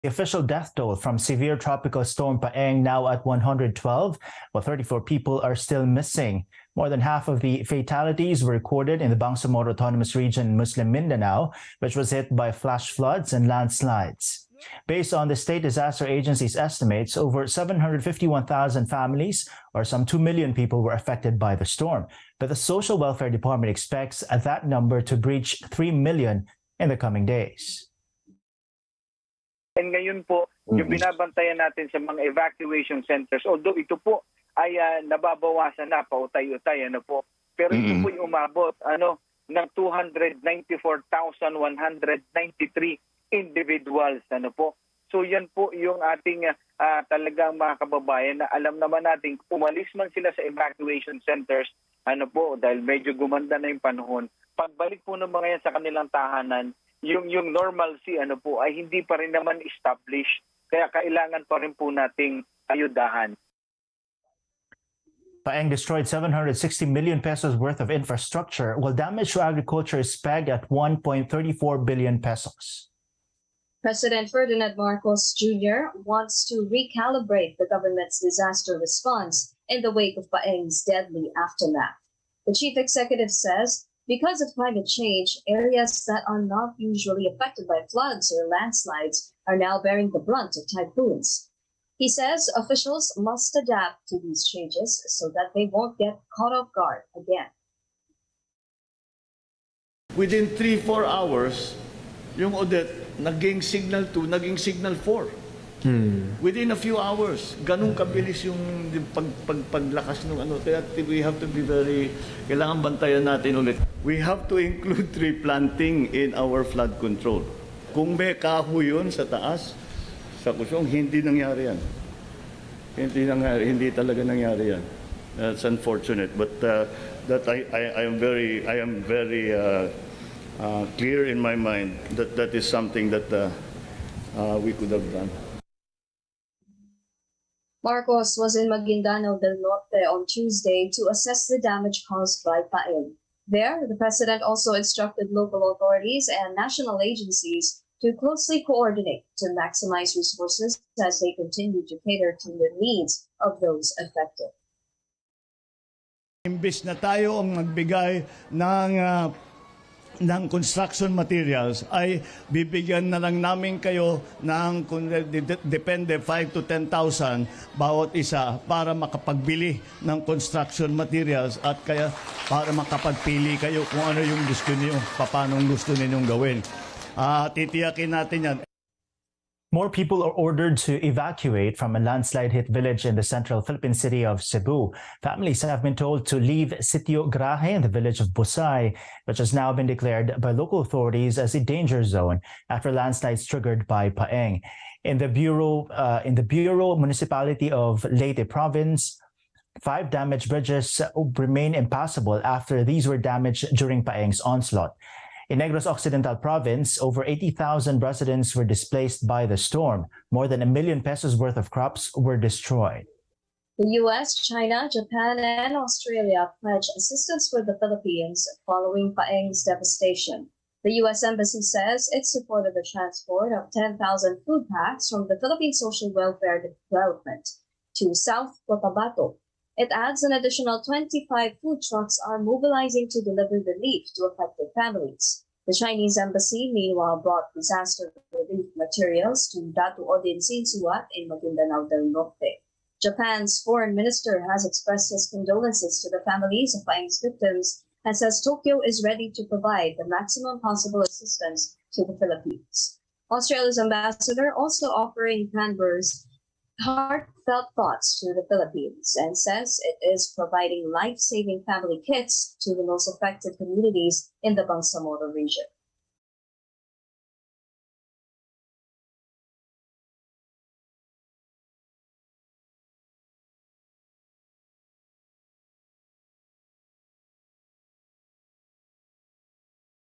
'Paeng' death toll rises to 112; 34 still missing | ANC
Rundown: The latest on the impact of 'Paeng'